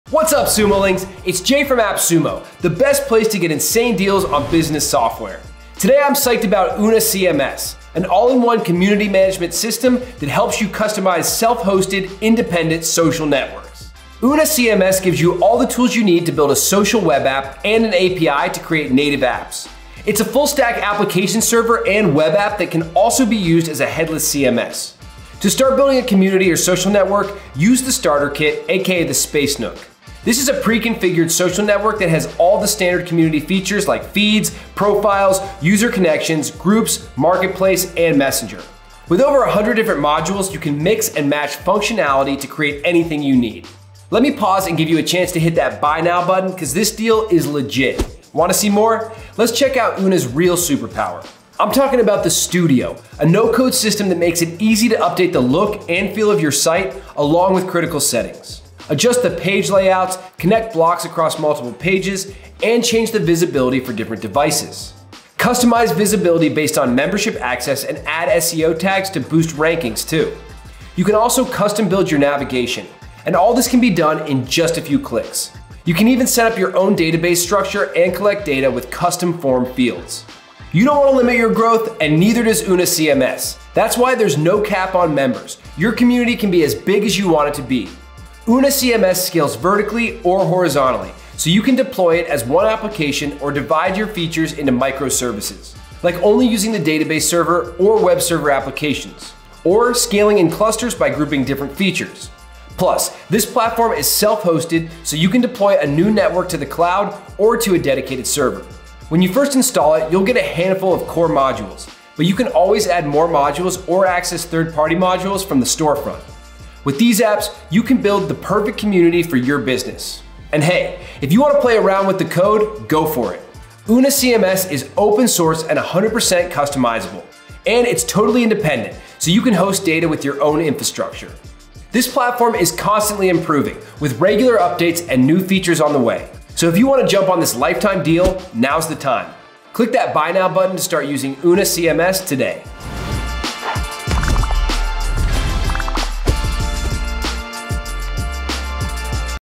Build Your Own Social Network with UNA CMS
UNA CMS is an all-in-one community management system that helps you customize self-hosted, independent social networks.

Build your own social network for your business using hundreds of no-code modules

You could connect with customers on Facebook and Instagram, but you won’t have control over anything. (“It’s Zuckerberg’s world, and we’re just living in it.”)

Between user privacy, data collection, and functionality, you’re at the mercy of Big Tech if you can’t build a social network from scratch.

Good thing there’s a tool that lets you launch and manage an independent online community with everything you need to scale your business.

Introducing UNA CMS.